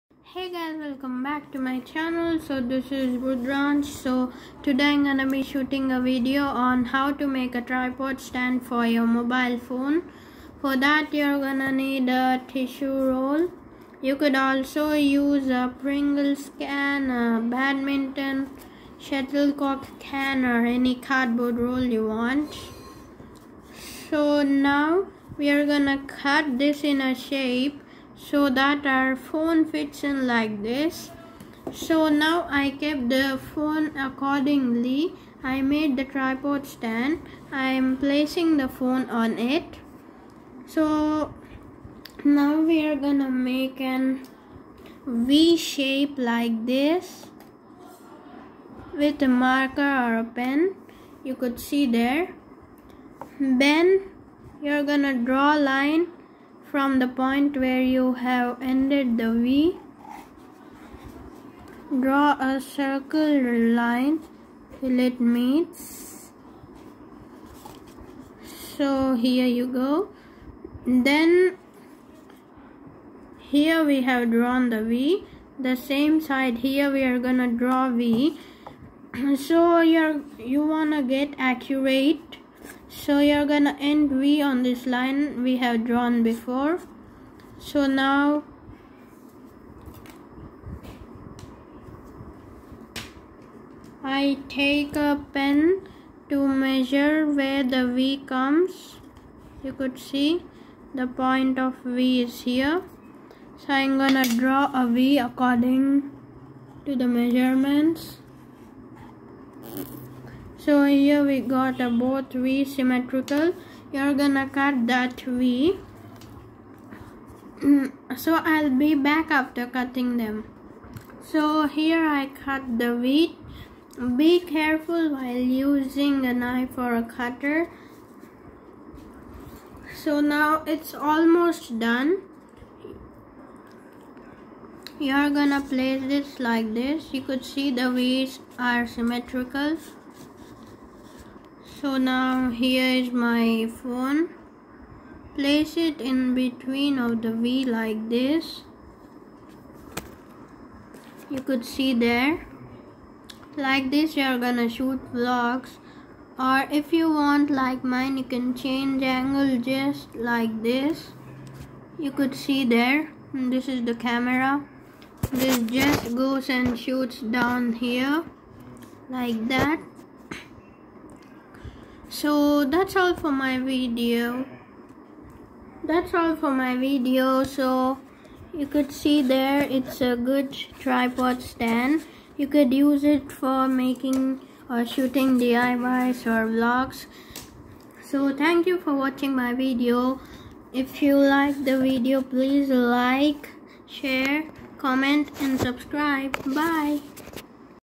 How to make homemade diy tripod stand for shooting diy's /vlogs (must watch!!)
hey guys ,i made a diy homemade tripod stand for diy/vlogs etc.,just using a cardboard roll.est out of waste.